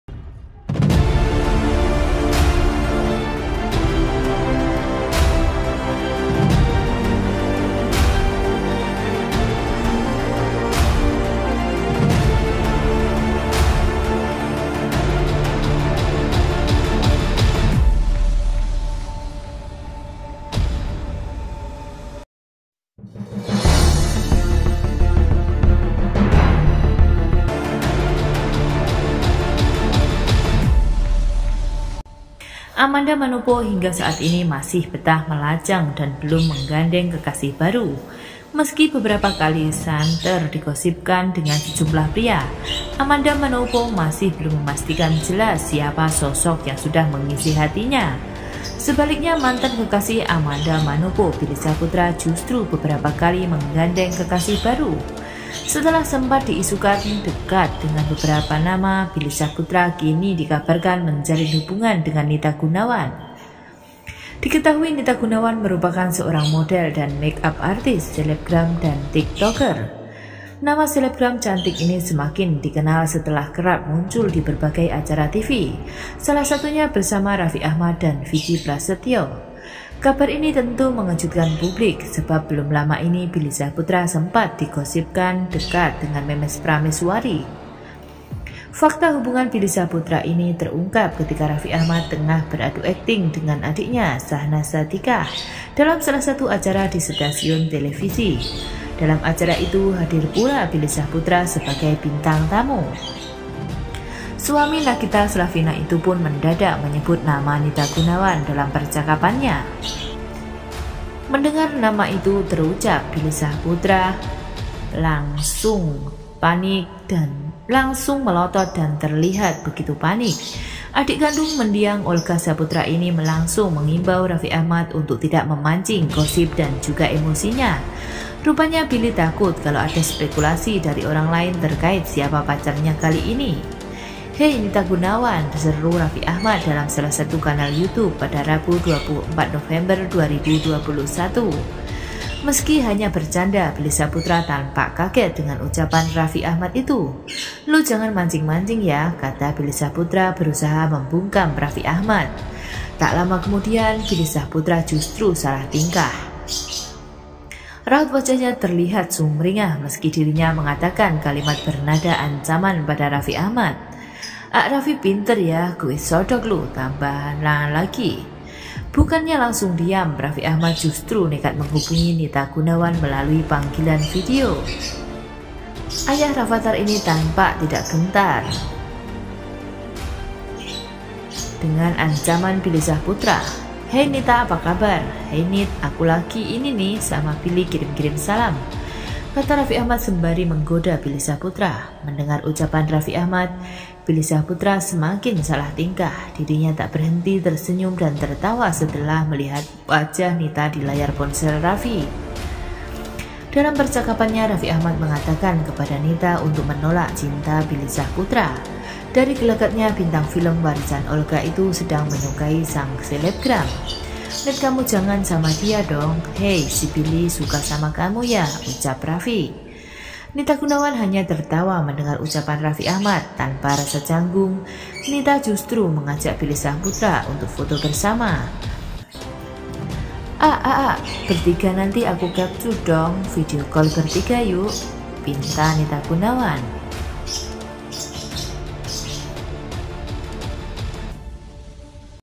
Billy Syahputra Sudah lupakan Masa lalu..Dan lagi Pendekatan Dengan Wanita Cantik ini#BillySyahputra
Nama Billy Syahputra biasanya akan dikaitkan dengan Amanda Manopo yang pernah menjadi pacar selama beberapa bulan.

Namun kini, kabarnya Billy sudah move on dari Amanda dan menjalin hubungan dengan Nita Gunawan.

Hal ini terungkap saat Raffi Ahmad tengah beradu akting dengan adiknya, Syahnaz Sadiqah dalam salah satu acara di stasiun televisi.

Di situ, ada juga Billy Syahputra. Suami Nagita Slavina itu mendadak menyebut nama Nita Gunawan dalam percakapannya.